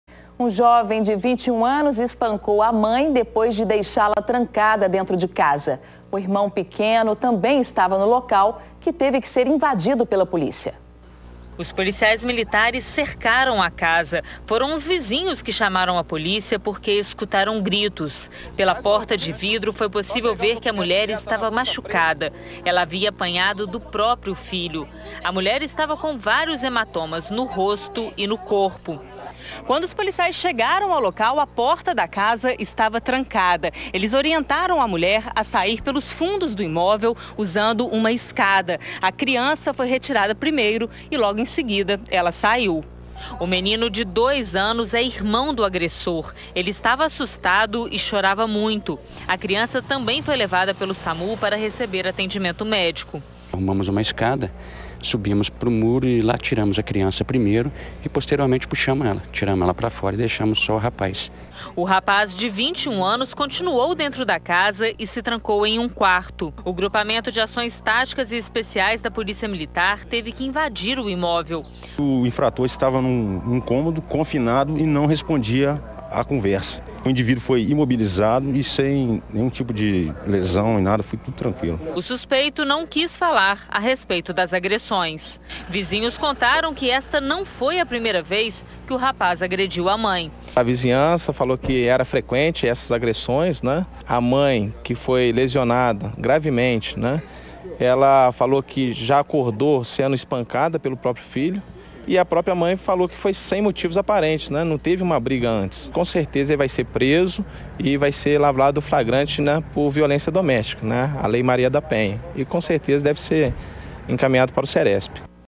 Jovem faz mãe e irmão reféns em Juiz de Fora
Um jovem de 21 anos fez sua mãe e irmão pequeno de reféns em Juiz de Fora. Ele ainda espancou a mãe depois de deixá-la trancada na casa, que teve de ser invadida pela polícia.